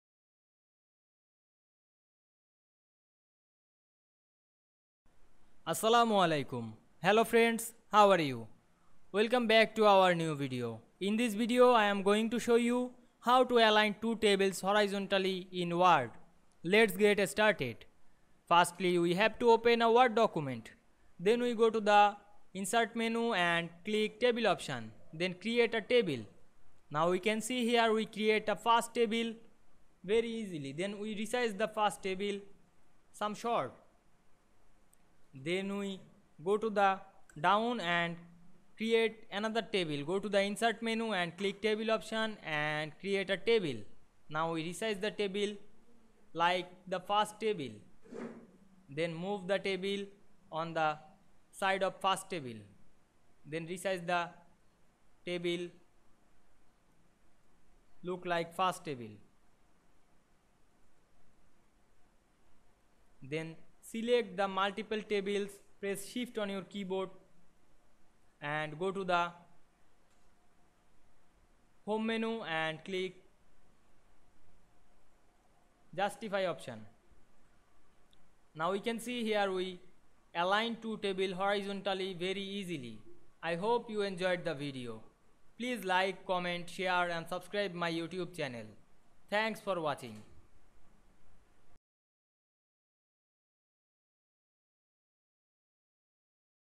How to align two tables horizontally in word
In this video I will show you, How to align two tables horizontally in word. Let's get started.